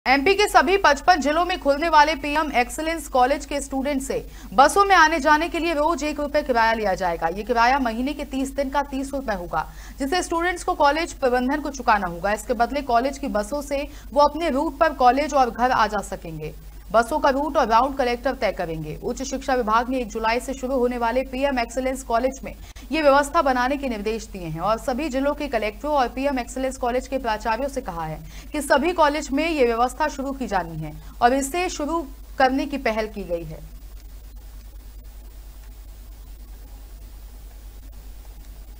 भोपाल - MP में एक रुपए में कॉलेज पहुंचेंगे स्टूडेंट
एमपी के सभी 55 जिलों में खुलने वाले पीएम एक्सीलेंस कॉलेज के स्टूडेंट्स से बसों में आने-जाने के लिए रोज एक रुपए किराया लिया जाएगा। यह किराया महीने के 30 दिन का 30 रुपए होगा, जिसे स्टूडेंट्स को कॉलेज प्रबंधन को चुकाना होगा। इसके बदले, वे कॉलेज की बसों से अपने रूट पर कॉलेज और घर आ जा सकेंगे। बसों का रूट और राउंड कलेक्टर तय करेंगे। उच्च शिक्षा विभाग ने एक जुलाई से शुरू होने वाले पीएम एक्सीलेंस कॉलेज में यह व्यवस्था बनाने के निर्देश दिए हैं। सभी जिलों के कलेक्टरों और पीएम एक्सीलेंस कॉलेज के प्राचार्यों से कहा गया है कि यह व्यवस्था सभी कॉलेजों में शुरू की जाए और इसके खर्च की भरपाई कॉलेज के जनभागादारी मद से की जाएगी। मुख्यमंत्री डॉ. मोहन यादव ने भोपाल में उच्च शिक्षा विभाग के अफसरों के साथ समीक्षा बैठक की, जिसमें सभी जिलों में खोले जाने वाले कॉलेज की व्यवस्थाओं का रिव्यू किया गया। बैठक में पीएम एक्सीलेंस कॉलेज में शुरू होने वाले पाठ्यक्रम और अन्य व्यवस्थाओं को पूरा करने के लिए सीएम ने अफसरों को निर्देशित किया।

In all 55 districts of MP, students of the upcoming PM Excellence Colleges will be charged a daily fare of one rupee for bus travel. This fare will be 30 rupees for 30 days in a month, payable to the college management. In return, students can travel to and from college on designated bus routes. The routes and rounds of the buses will be decided by the collector. The Higher Education Department has instructed that this system be implemented in PM Excellence Colleges starting from July 1. All district collectors and principals of PM Excellence Colleges have been asked to initiate this system, with the costs covered by the college's participation fund. Chief Minister Dr. Mohan Yadav conducted a review meeting with higher education officials in Bhopal, reviewing the arrangements for the colleges to be opened in all districts. During the meeting, the CM directed officials to complete the courses and other arrangements for the PM Excellence Colleges.